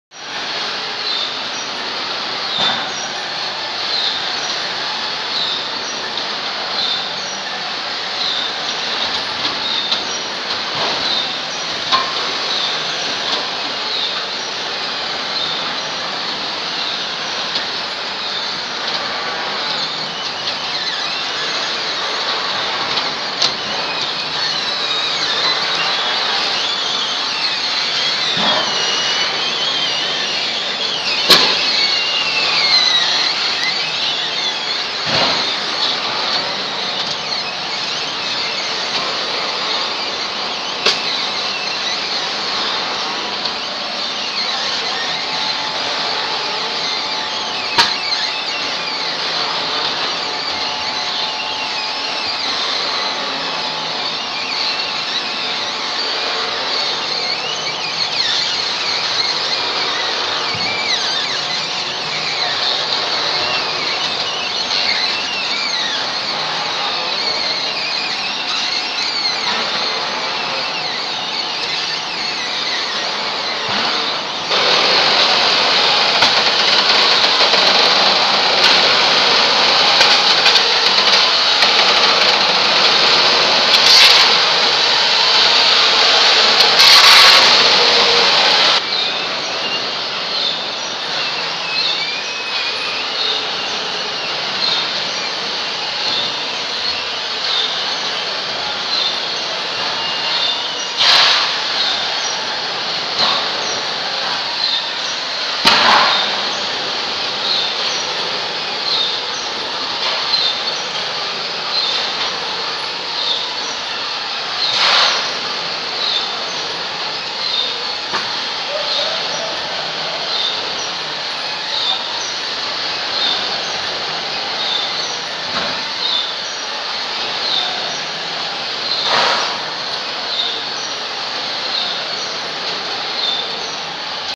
VIDEO OF ELEVATOR WORKING
Palm kernel nuts elevator in operation at a crushing factory.